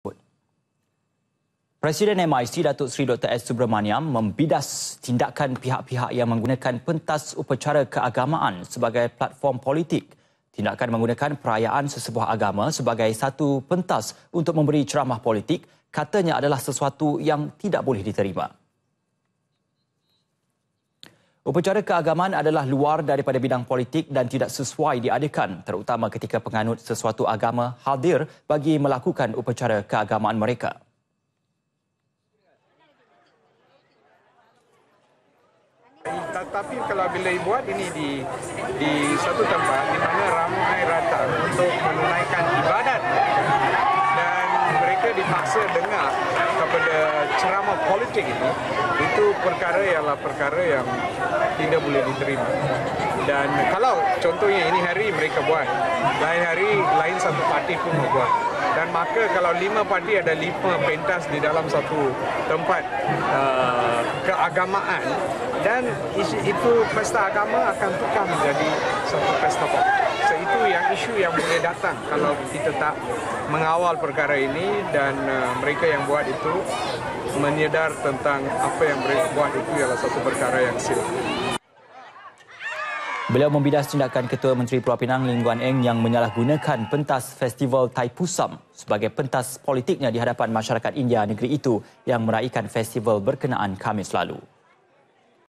PRESIDEN MIC: SALAH GUNA AGAMA, TINDAKAN MANIPULASI DAP DIKESALI [12 FEB 2017]
Presiden MIC, Datuk Seri Dr. S. Subramaniam membidas tindakan pihak-pihak yang menggunakan pentas upacara keagamaan sebagai platform politik.

Tindakan menggunakan perayaan sesebuah agama sebagai satu pentas untuk memberi ceramah politik katanya adalah sesuatu yang tidak boleh diterima.